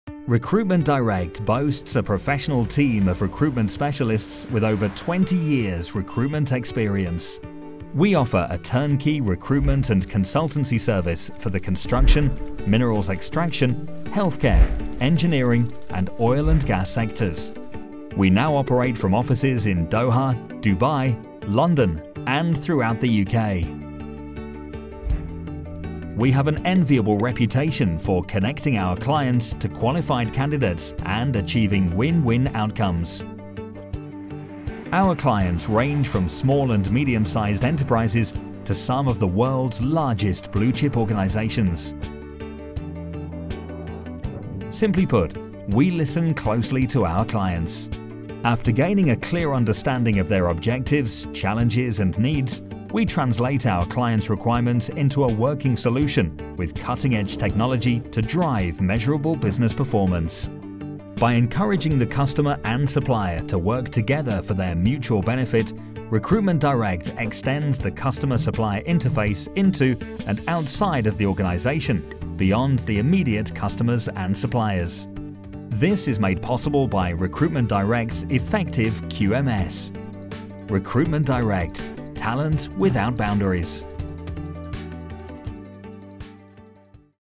Recruitment Direct Overview- Client Promo Video
This video combines motion graphics with stunning imagery to create a film used at conferences, events and online.  The script was developed by our client and we created the right video to help them target new business in the middle east.